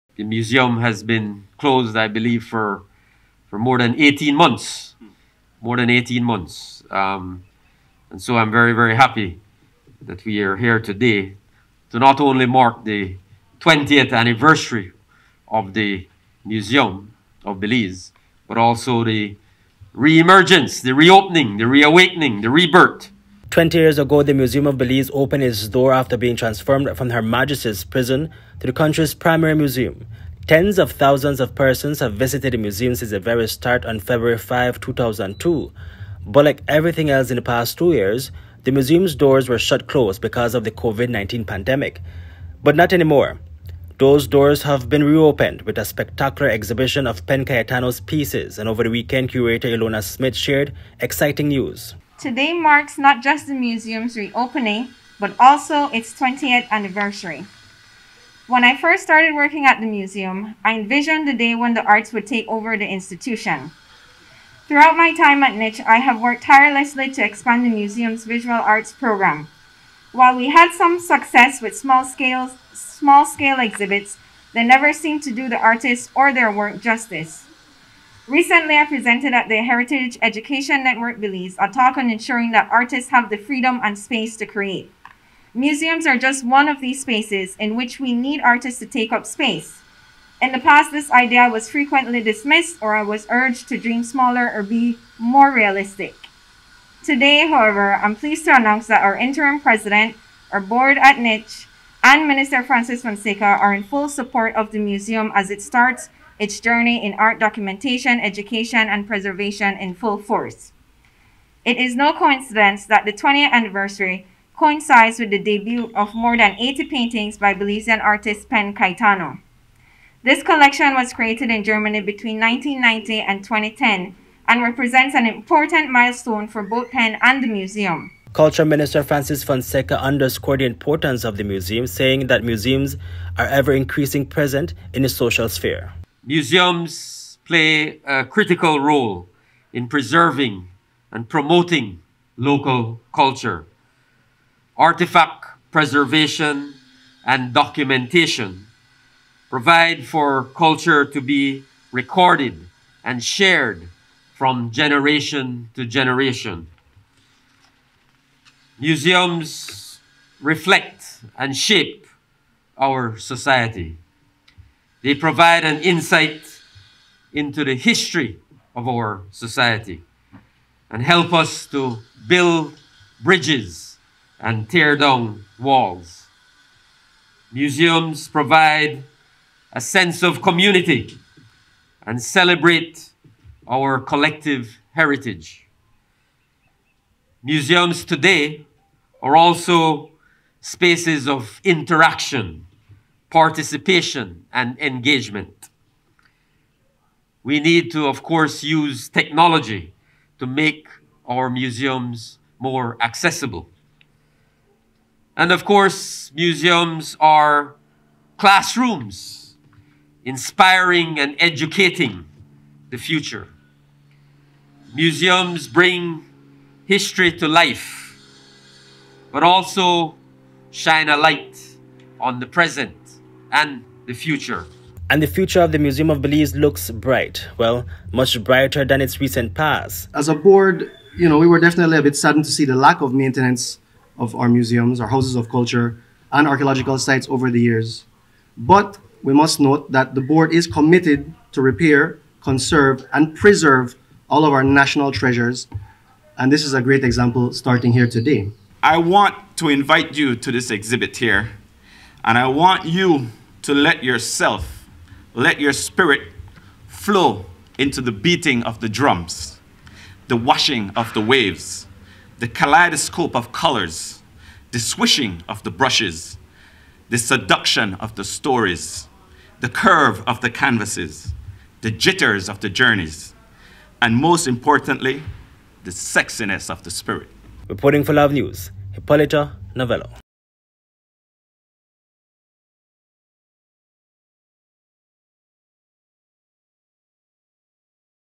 The Museum of Belize Celebrates its 20th Anniversary
After being closed for almost two years, the Museum of Belize officially re-opened its doors to the public on Saturday. The museum also celebrated its 20th anniversary with a specular exhibit of Pen Cayetano’s pieces. Senior Reporter Hipolito Novelo has the story.